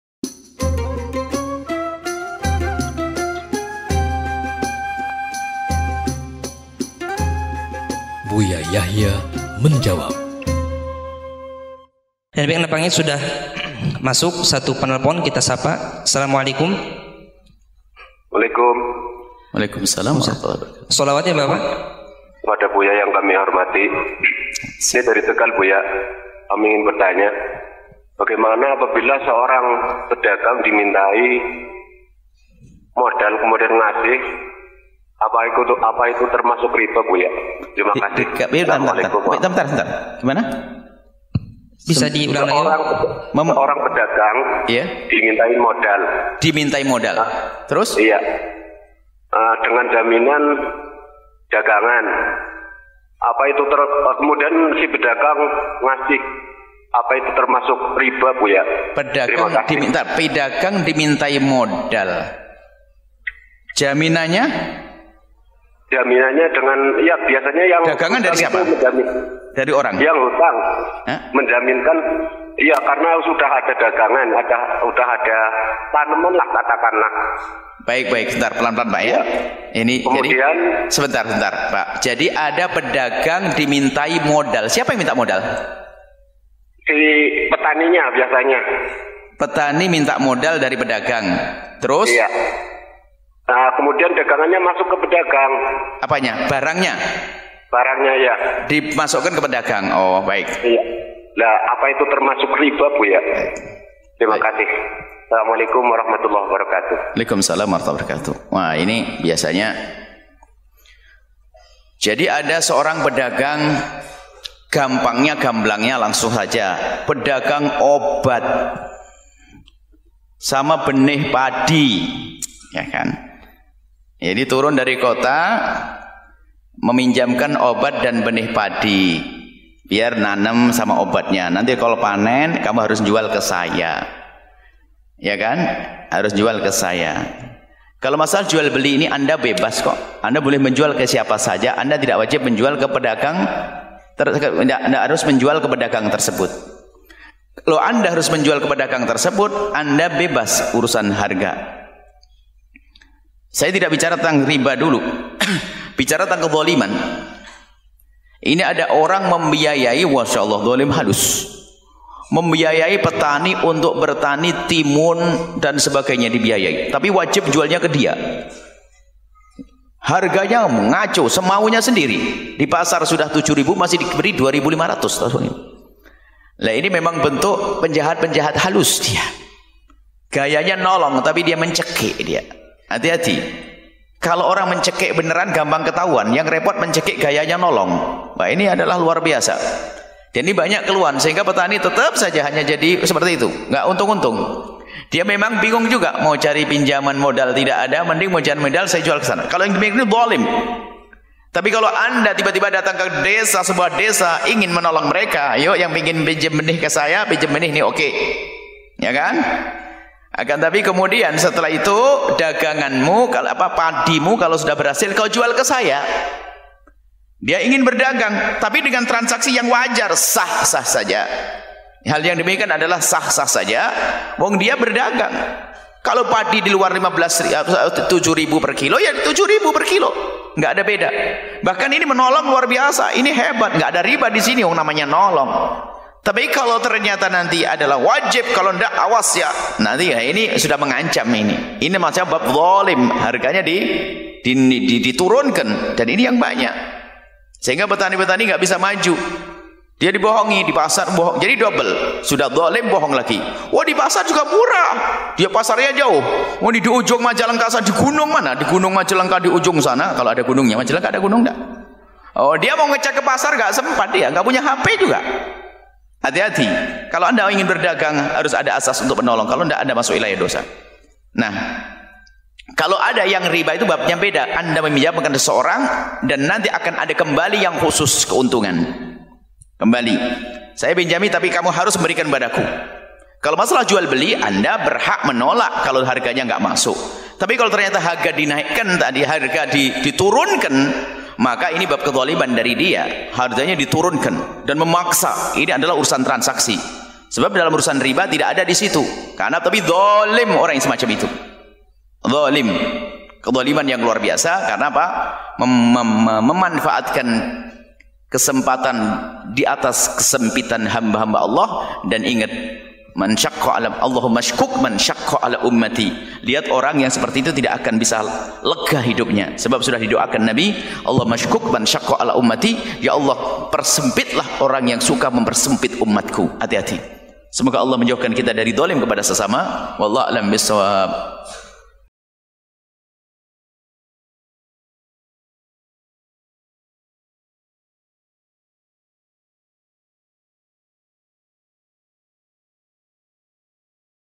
Pedagang dimintai modal oleh petani dengan jaminan dagangan, apakah riba? | Buya Yahya Menjawab
Pedagang dimintai modal | Buya Yahya Menjawab